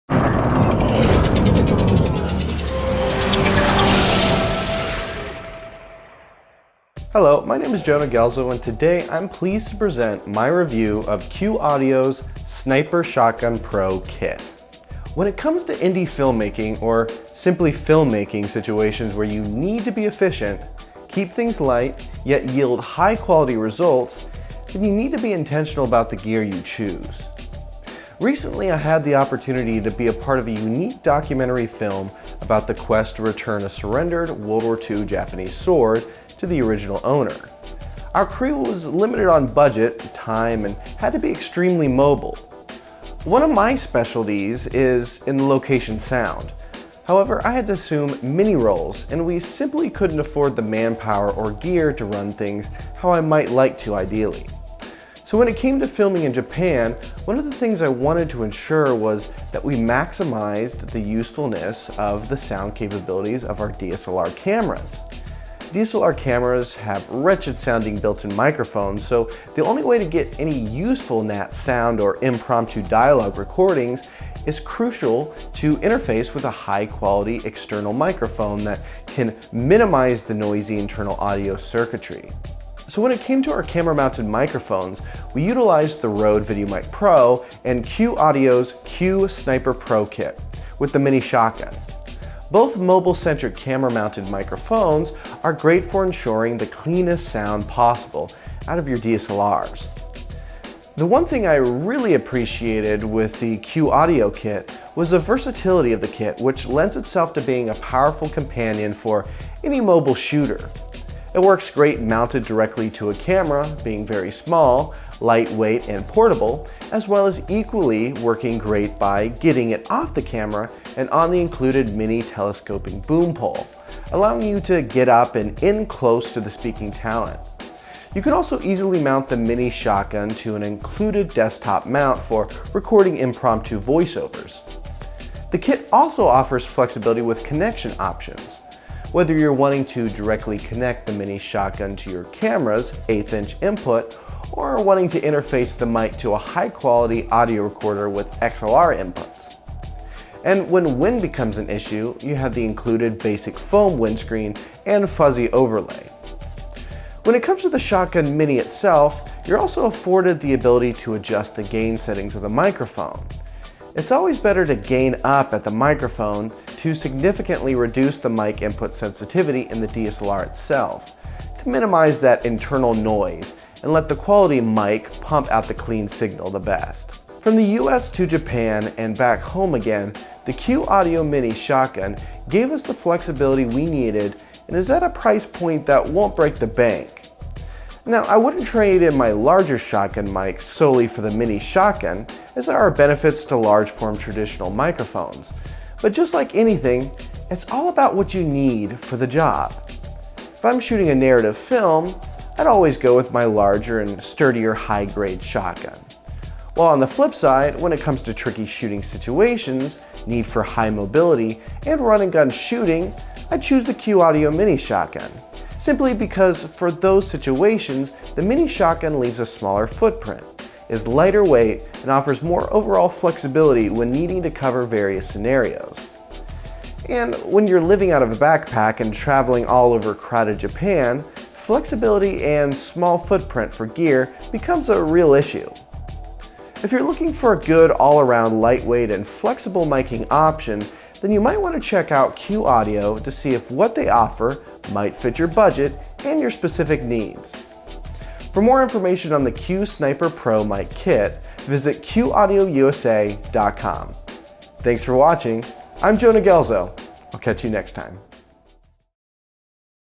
QUE AUDIO: Q Sniper Pro Microphone Kit REVIEW
I got the chance to really put the Que Audio Q Sniper Pro Mic Kit to the test in an overseas documentary in Japan. Explore the richness of this portable mics versatility and how it might help you when it comes to run and gun shooting applications.